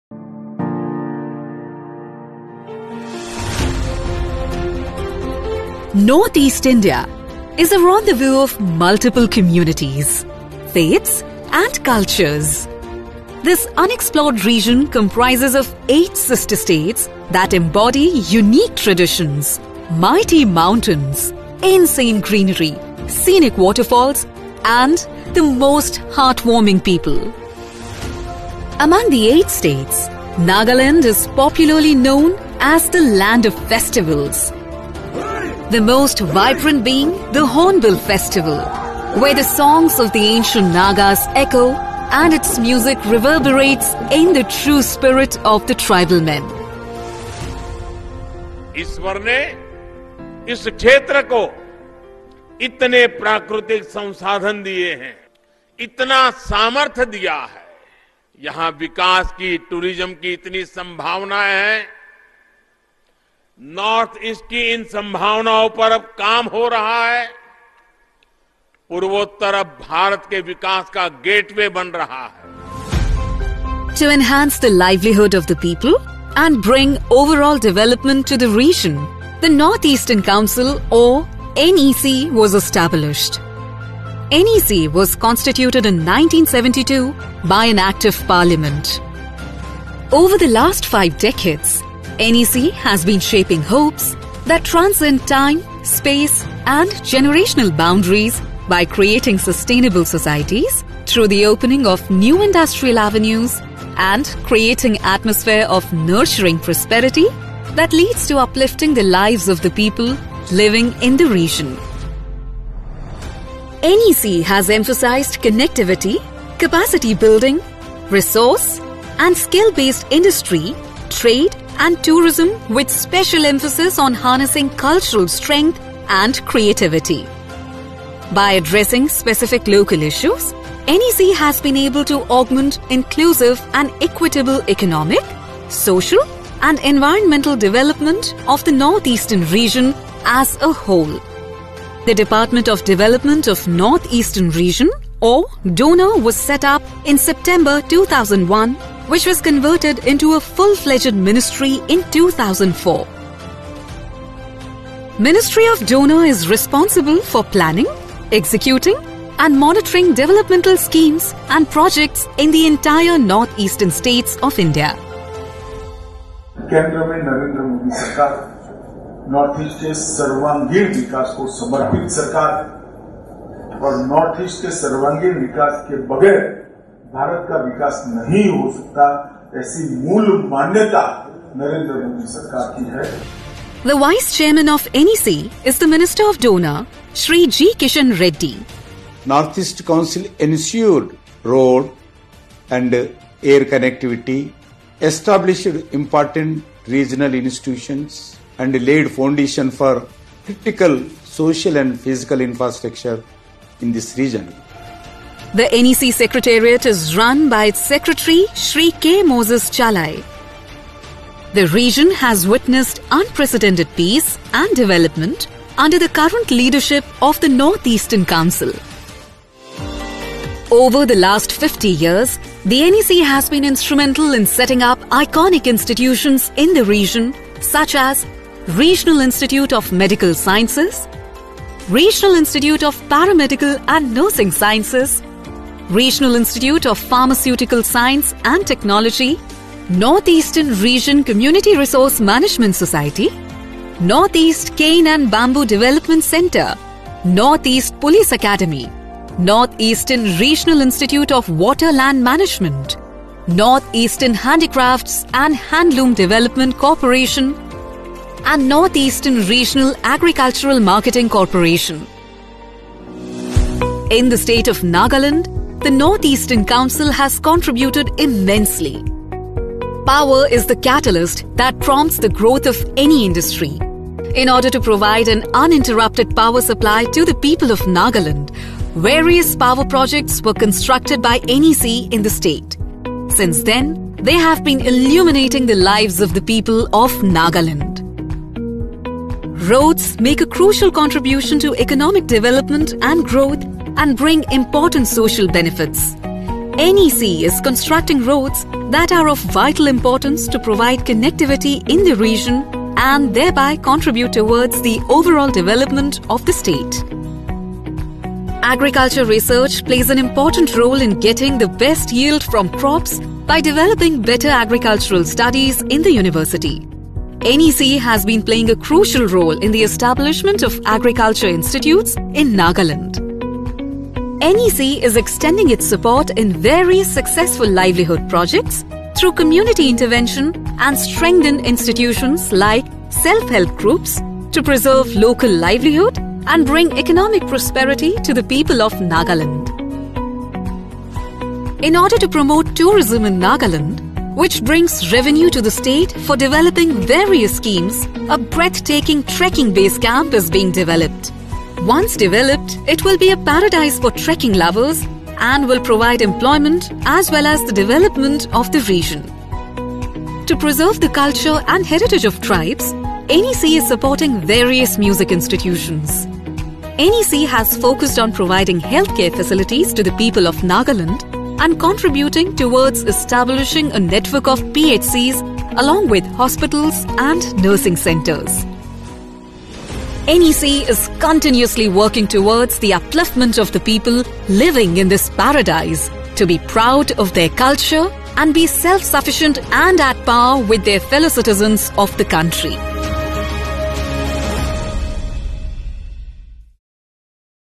Towards Progress, Growth & Development of NorthEast🇮🇳Nagaland
Towards Progress, Growth & Development of NorthEast🇮🇳

Nagaland, 'The Land of Festivals' is known for their vibrant festivals celebrating age old tradition and culture.

The NEC with support from @MDoNER_India is committed to developing the state of Nagaland with power infrastructure, roads and connectivity, agriculture research and institutes, livelihood projects through SHGs, tourism development, health and music development, etc

NEC is continuously empowering communities through progress and development in the NorthEast